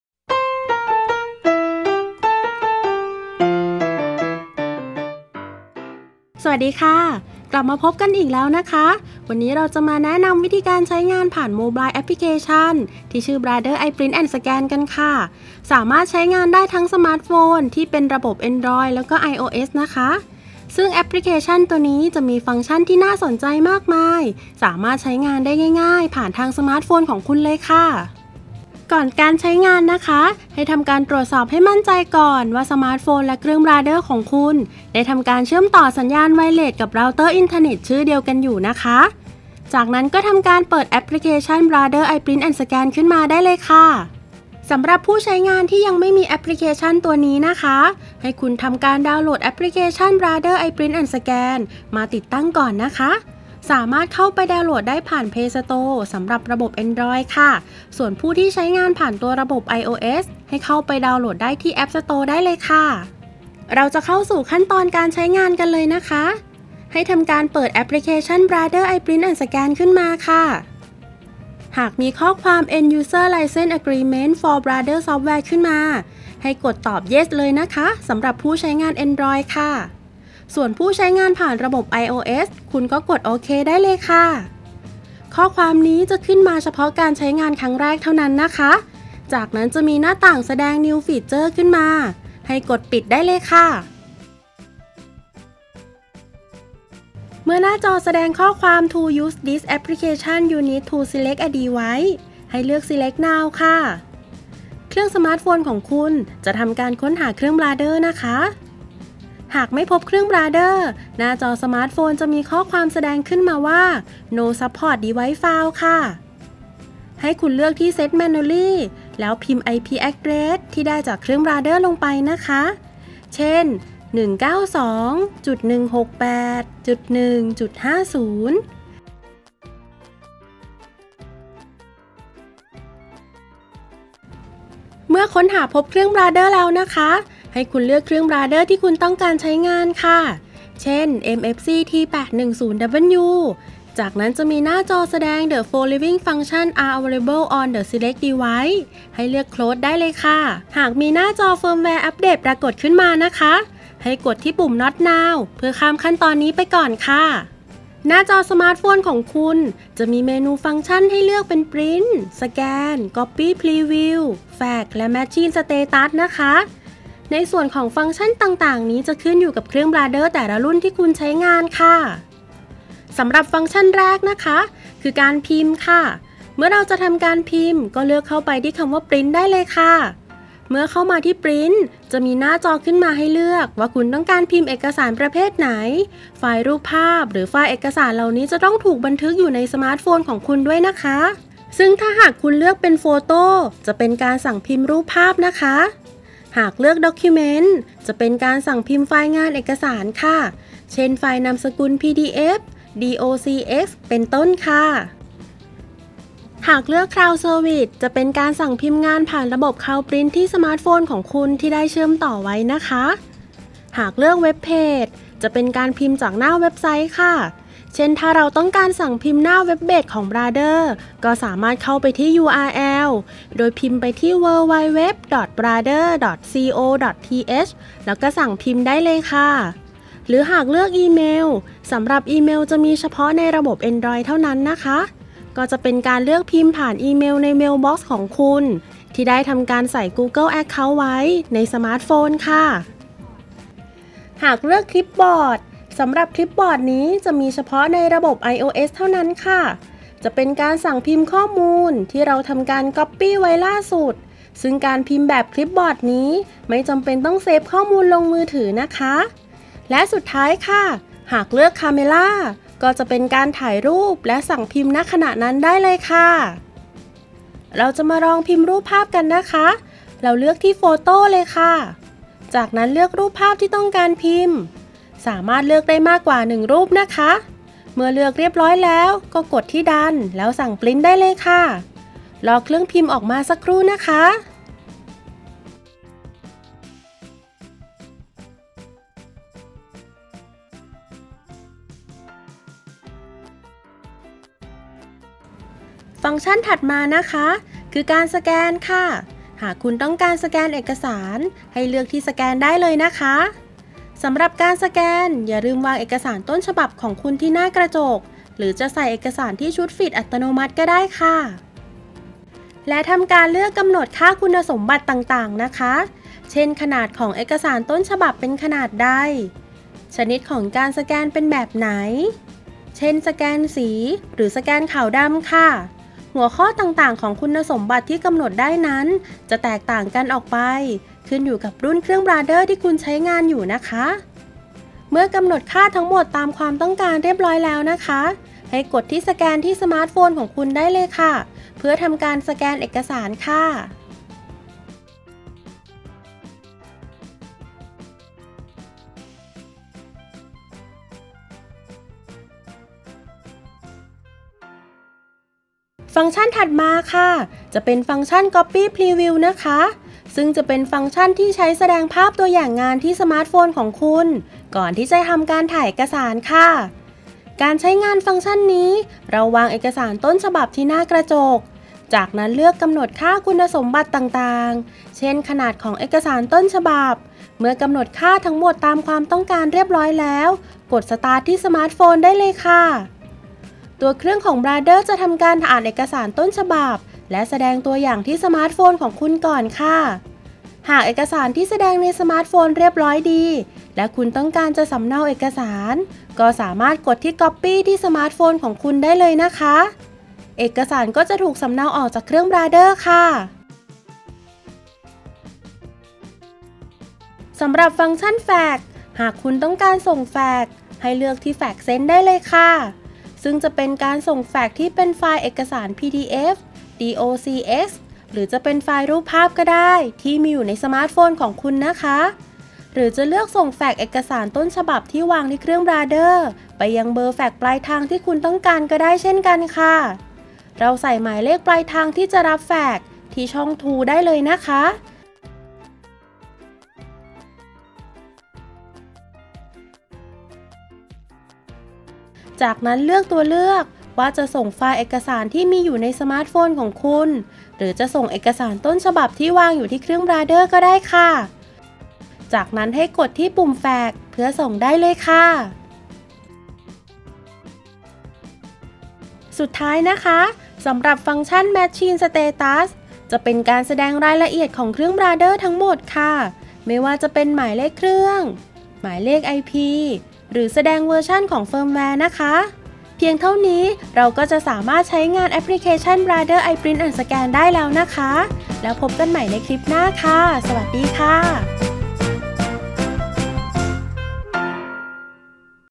การใช้งาน Mobile Application - Brother iPrint&Scan
เราสามารถใช้งานเครื่องบราเดอร์ได้ง่ายๆ ผ่าน Mobile Application       Brother iprint&Scan มีฟังก์ชั่นหลากหลายให้เลือกใช้งานนะคะ